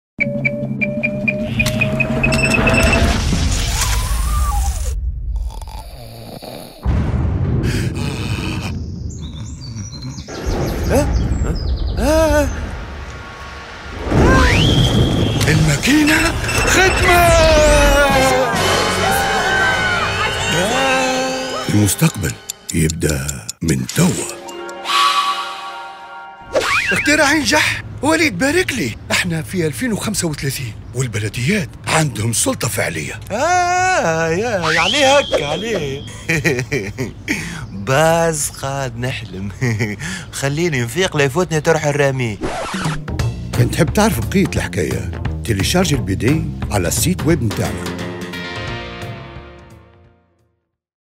(3) الماكينة : نفعلو اللامركزية ونشدو الثنية
Cette Bande Dessinée est un travail collectif, partant d'une initiative:
Propsée par Mme Samira Nefzi
Révisée par Nidhal Attia et Olfa Chebaane
Conçue et réalisée par l'équipe de "Le Monde de Kadar": Direction artistique: Kadar Nefla, Dessin: Chakib Daoued et Scénario: Imen Kadachi & Amin Ghazouani.

► Suivez-nous!